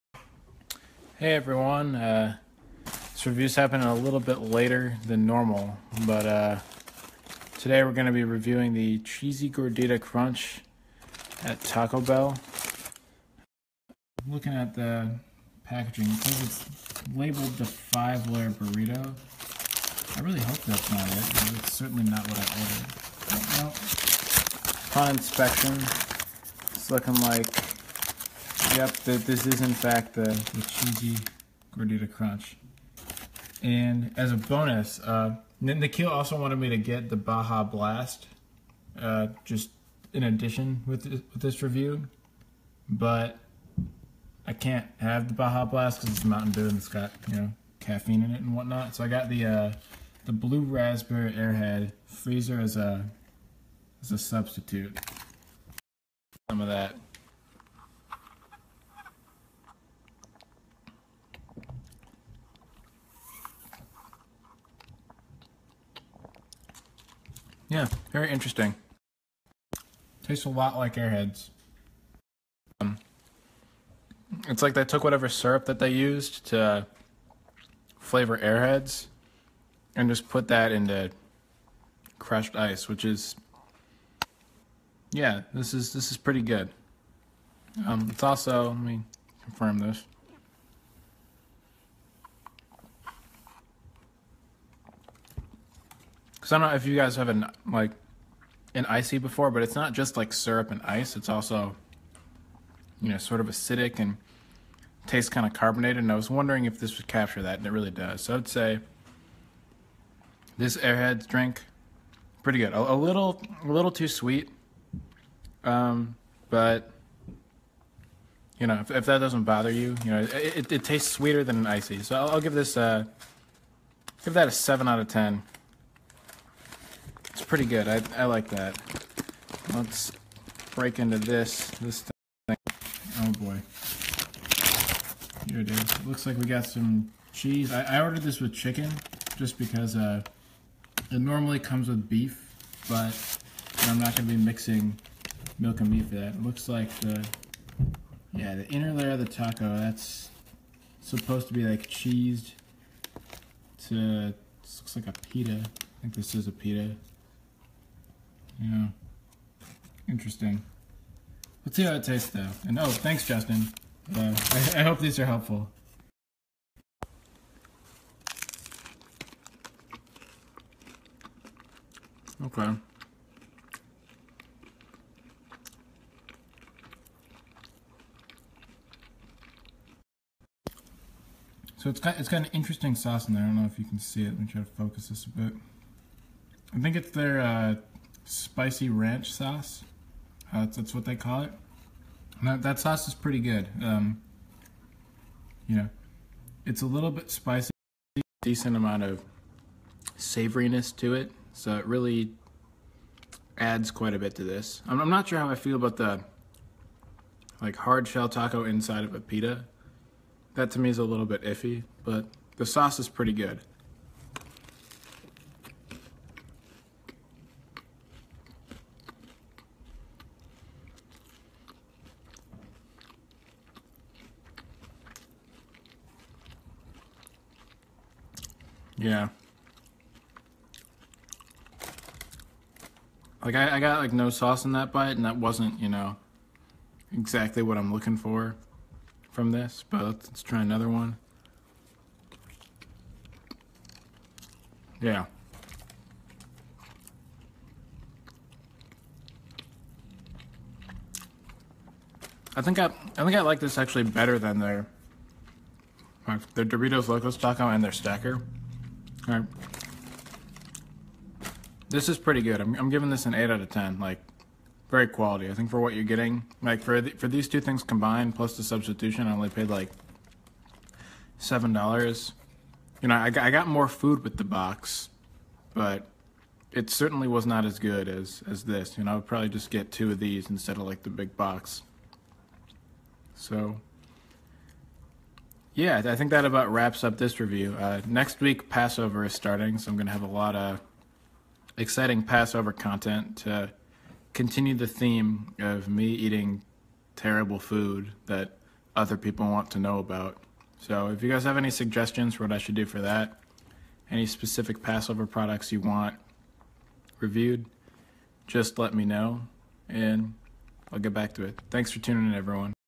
Taco Bell Cheesy Gordita Crunch Review
Today I try the Cheesy Gordita Crunch and the Blue Raspberry Airhead Freezer. Does the Spicy Ranch sauce live up to its reputation?